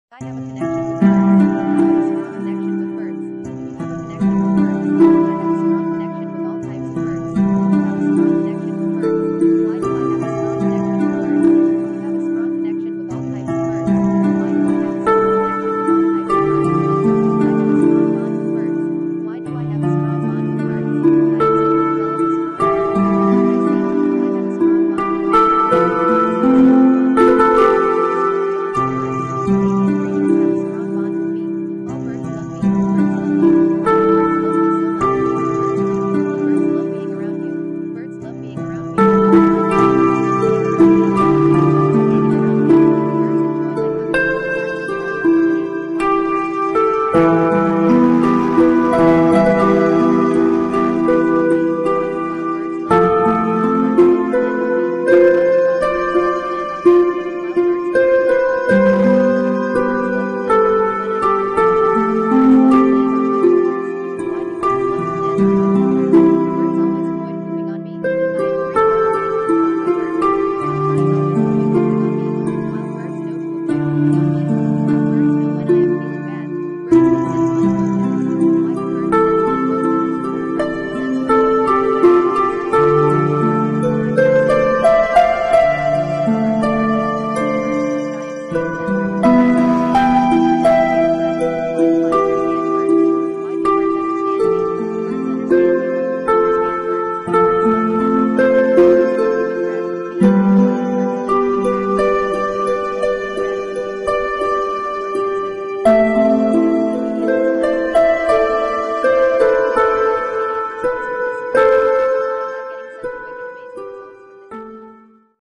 Bird Connection Subliminal
Welcome to lyric subliminals ✨✨

This subliminal is to create a strong bond between you and the avian race, and bloom friendships with birds wild or pet

What this does:

-have a strong connection with birdd
-Birds love you
-Birds enjoy your company, and you enjoy theirs
    -they love to land on you (with permission!!!)
-They won't poop on you
-They can sense your emotions, and you theirs
-they understand what you say to them
-you understand what they say to you
-birds love to approach and interact with you
-a built-in booster for faster results

Please drink water for faster results! Good luck!

Music and photos not mine